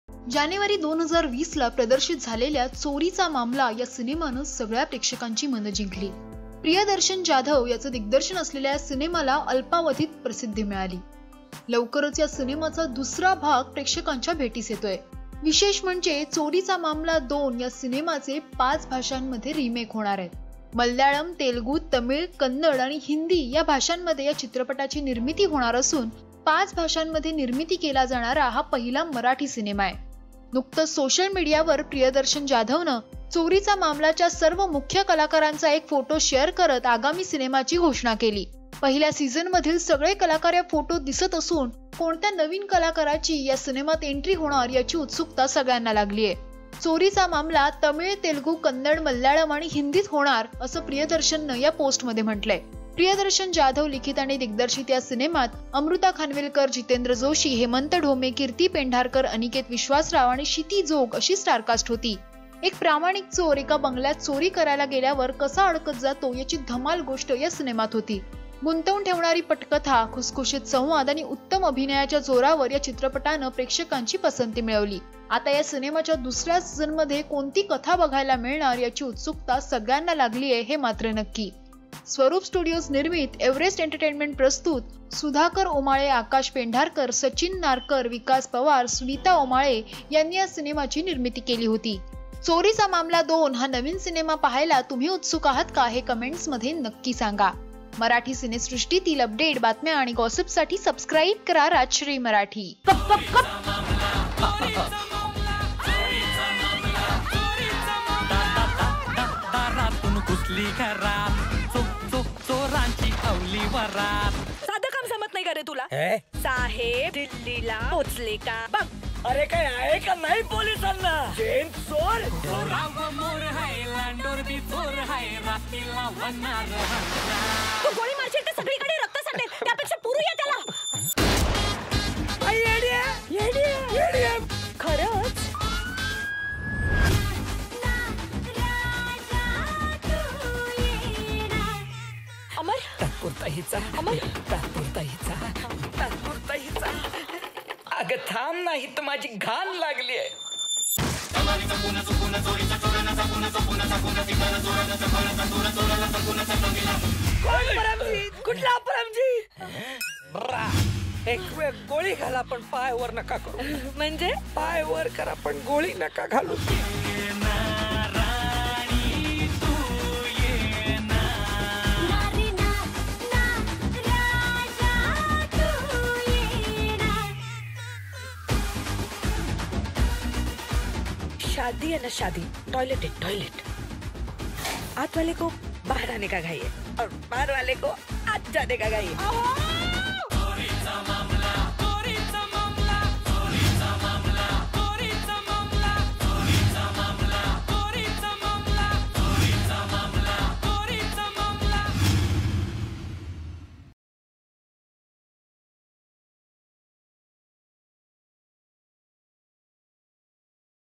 Choricha Mamla Part 2 ANNOUNCED | आता येतोय चोरीचा मामला २ | New Marathi Movie 2020
चोरीचा मामला या हिट सिनेमानंतर दिग्दर्शक प्रियदर्शन जाधव आता या सिनेमाचा दुसरा पार्ट लवकरच घेऊन येतोय. जाणून घेऊया या सिनेमाविषयी या व्हिडिओमध्ये.